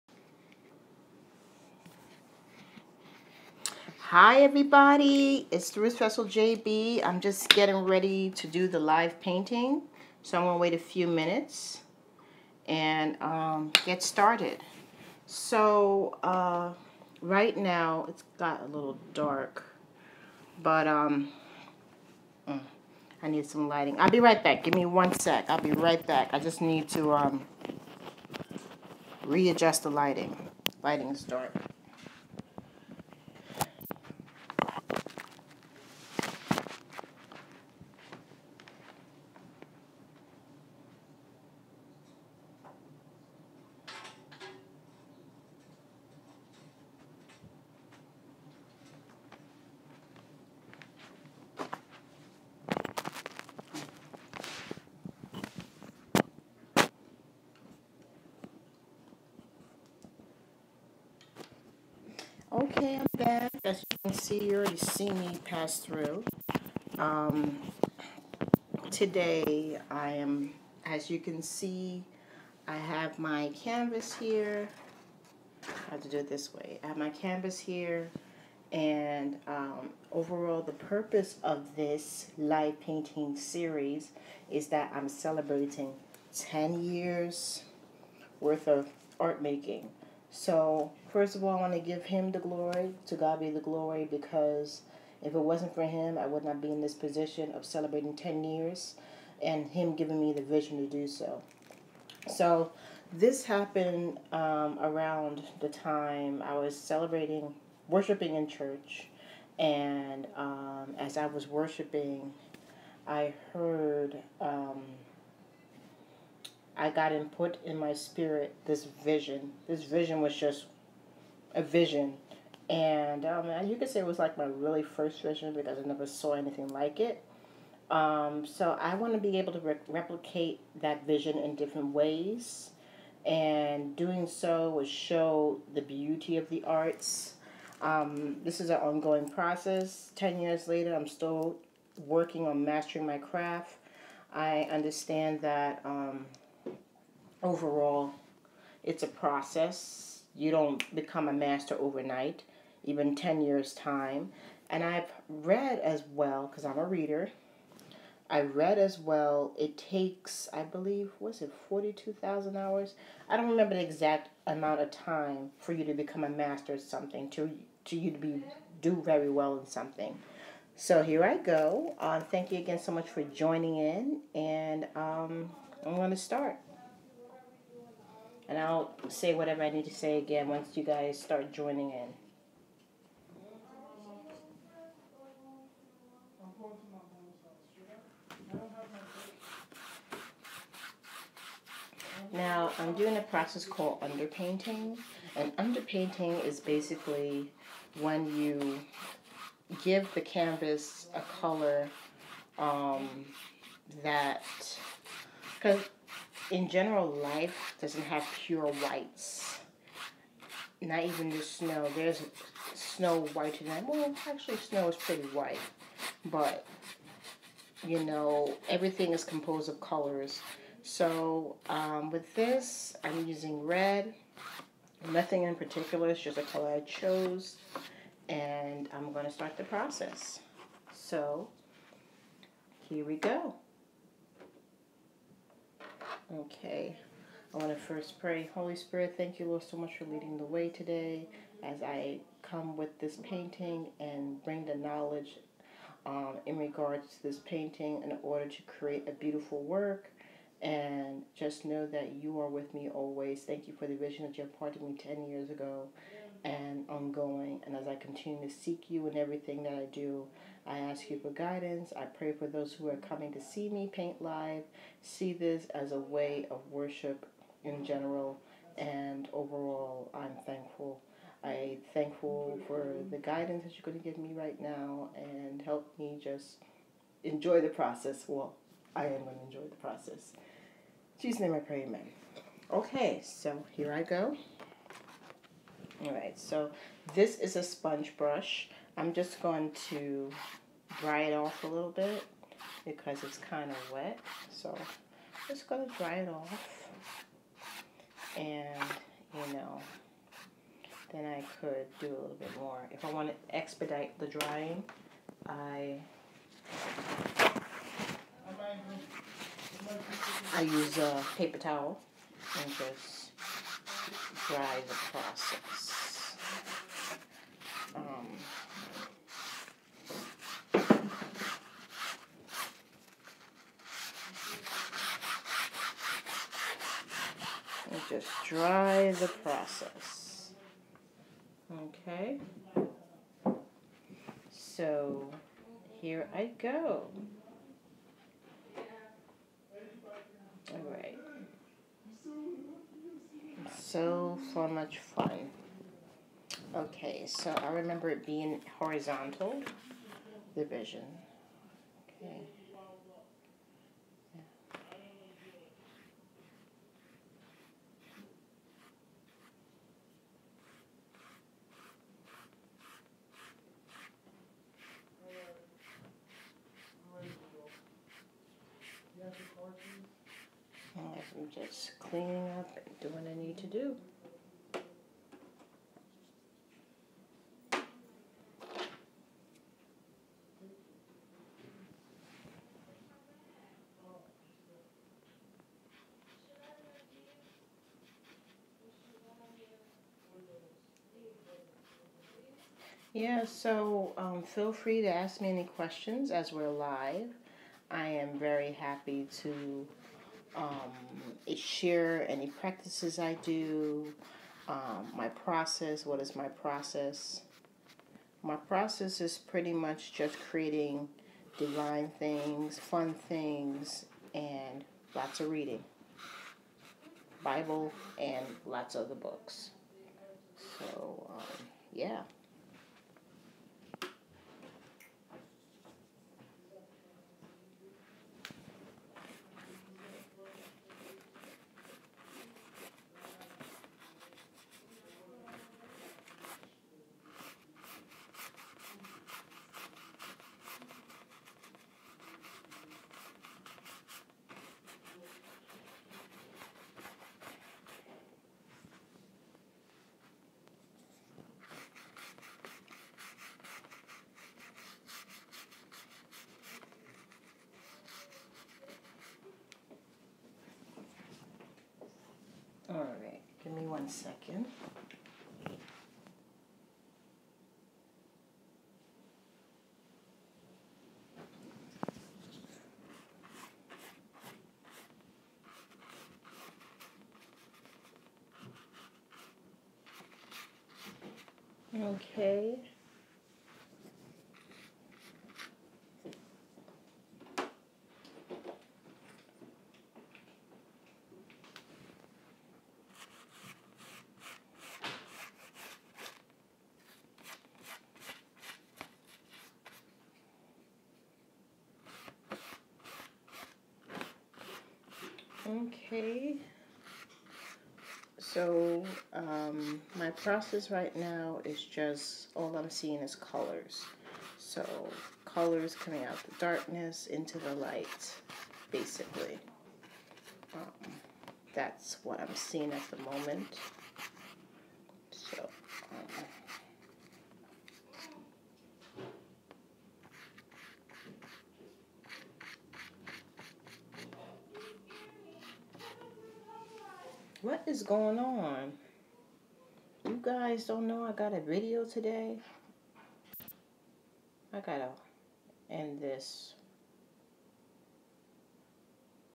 Thru His vessel JB celebrates 10 years of artmaking!
Thru His vessel JB celebrates 10 years of art making starting July 1 2019 via Instagram live. All paintings are available for sale and post sales benefit YLS Events, Paint your blessings, an organization that gives free painting classes monthly to disadvantaged individuals in Brooklyn.